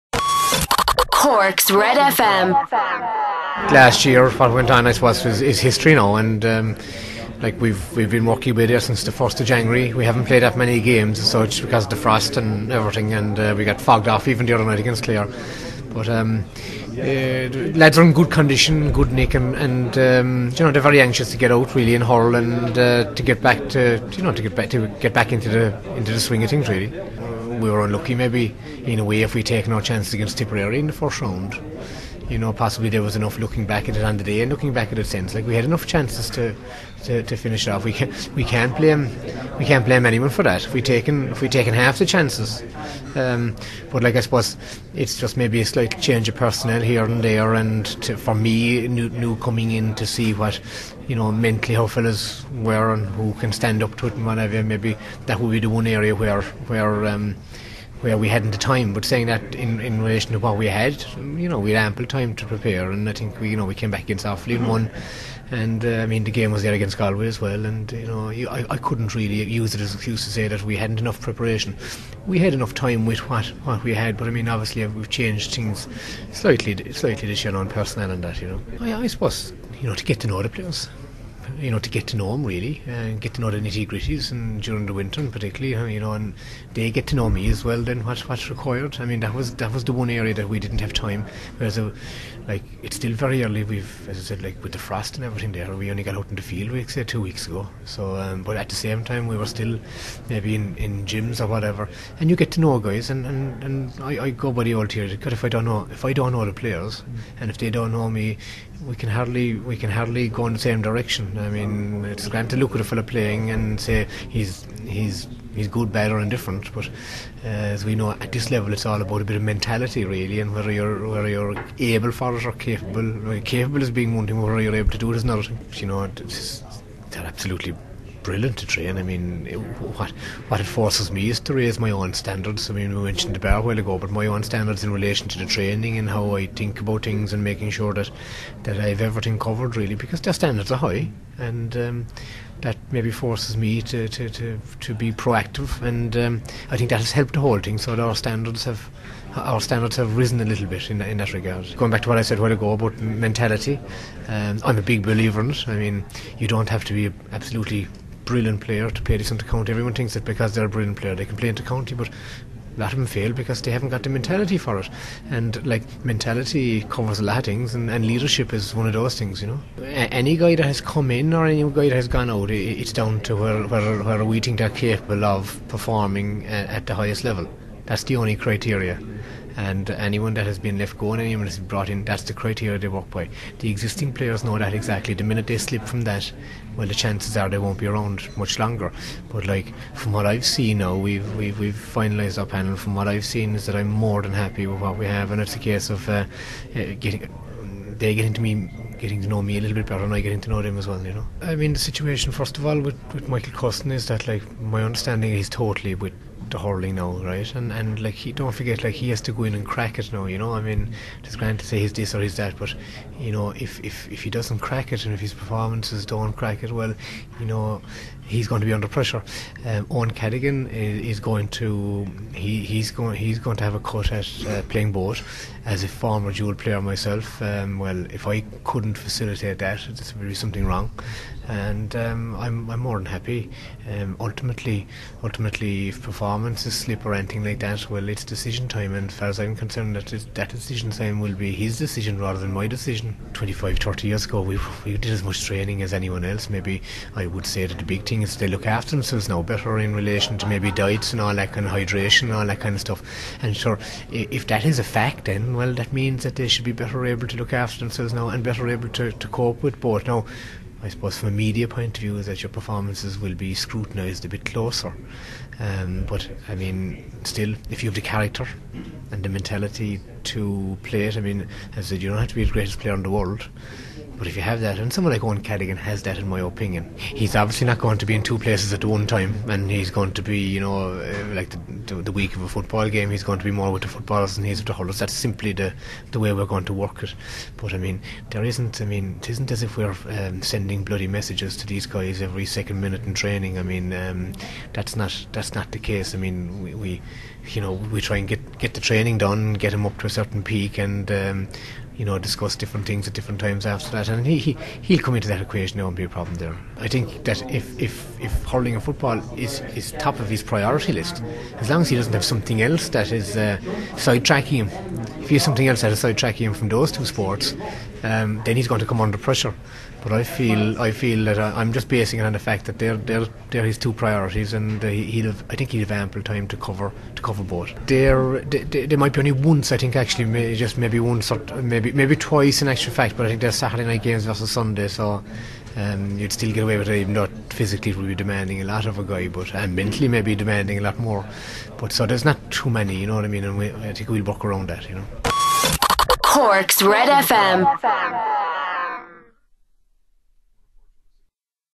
Denis Walsh 140210
Cork hurling manager Denis Walsh looking ahead to the start of the National League and the 2010 season.